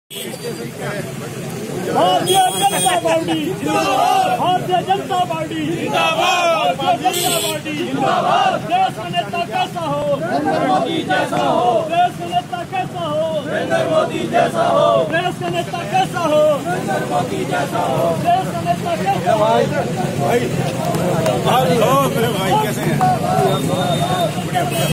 माजरा थाने में उड़ाई गयी आदर्श आचार संहिता की धज्जियां
माजरा थाने के अंदर भाजपा प्रत्याशी सुरेश कश्यप व राजीव बिंदल के द्वारा सरेआम उड़ाई गयी  आदर्श आचार संहिता की धज्जियां लगाये गए भाजपा के पक्ष में नारे